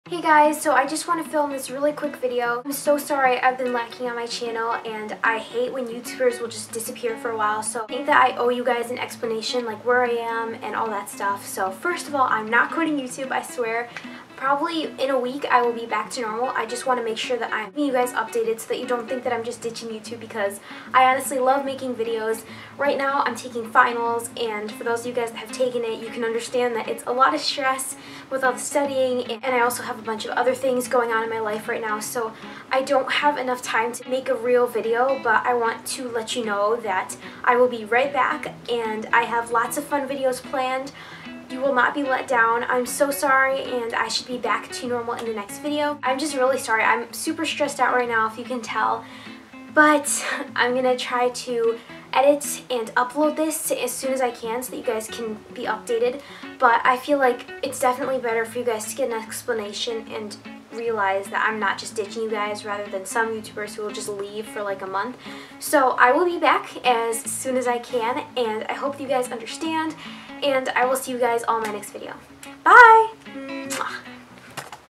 I'LL BRB(:
I hope you understand haha finals are the absolute worst when it comes to uploading and balancing a social life! I'll be back next week haha love you guys!